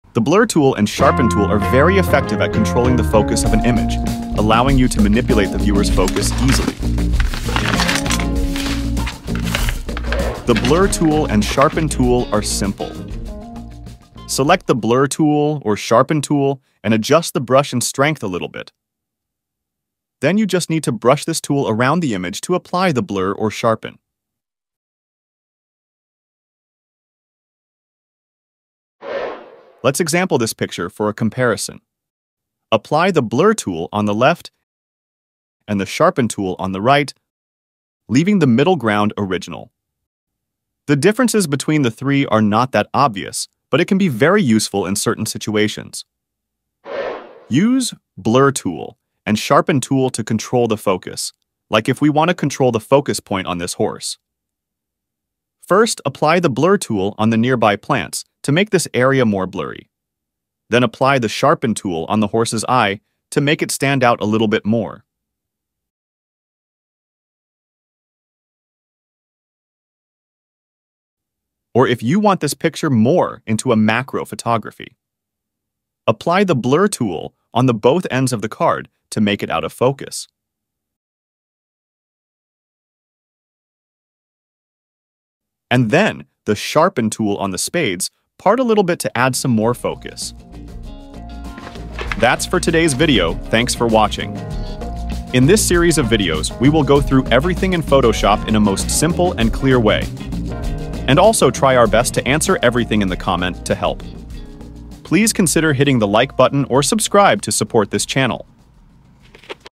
Blur Tool & Sharpen Tool - Toolbar Explained & Demonstrated [Photoshop Tutorial for Beginners]
The “Blur Tool” and “Sharpen Tool” are very effective at controlling the focus of an image, allowing you to manipulate the viewer’s focus easily.

0:11 How to use Blur Tool & Sharpen Tool
0:31 Blur & Sharpen Comparison
0:48 To Control Focus